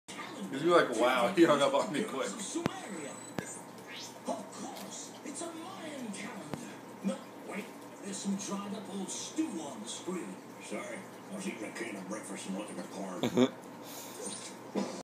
Convict stealing food
This little guy just can't keep out of trouble. Here he is stealing a bigger fish's food which he can't even eat himself!